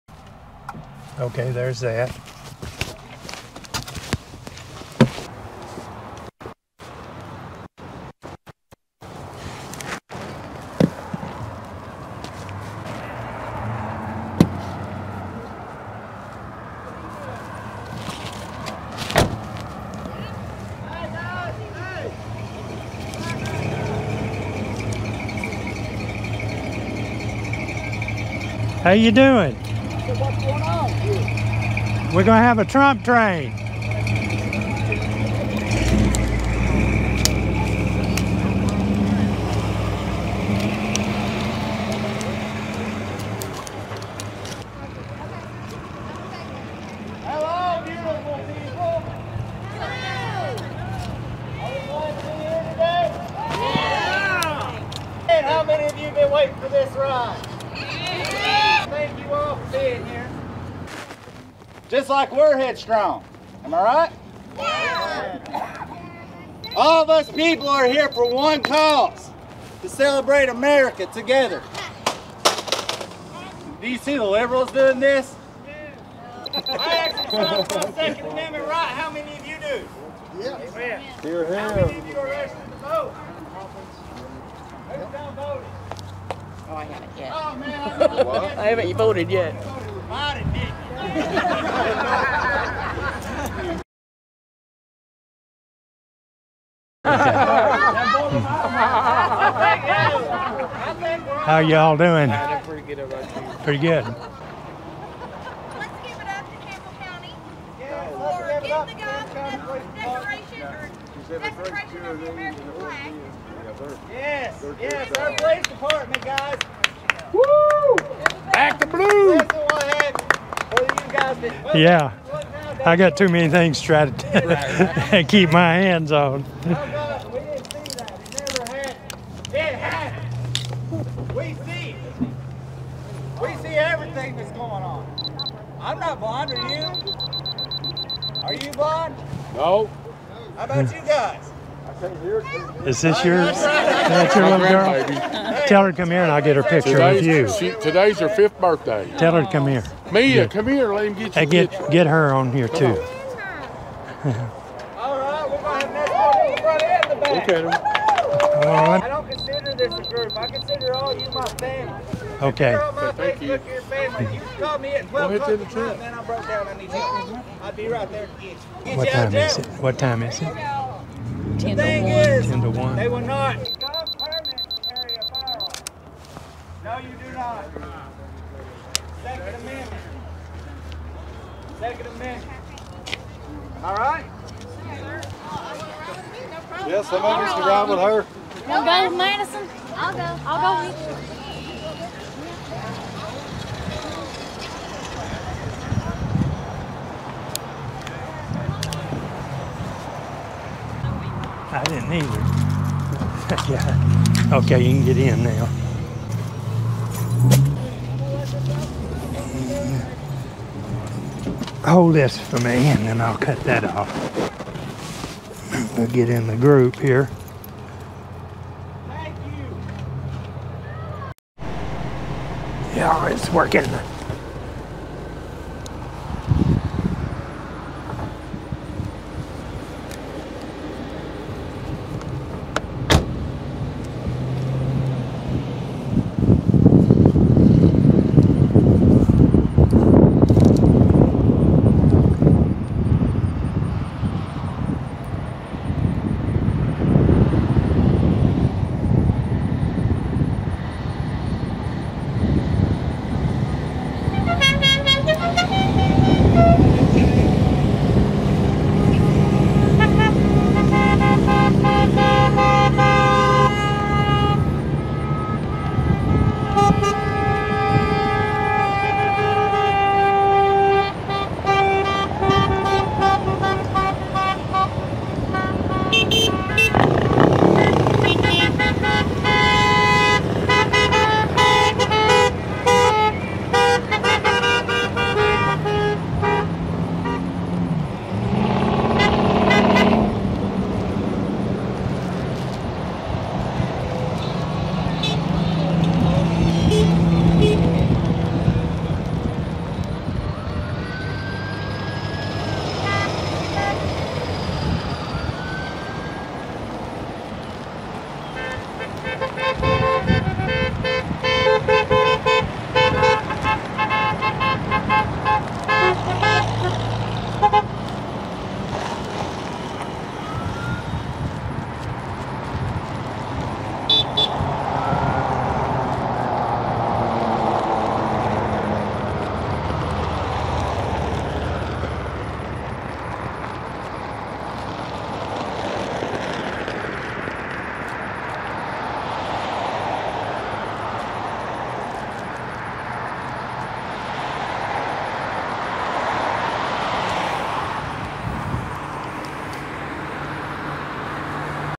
TRUMP TRAIN CAMPBELL COUNTY TENNESSEE IN 4K
A GROUP GOT TOGATHER TO SUPPORT PRESIDENT DONALD J TRUMP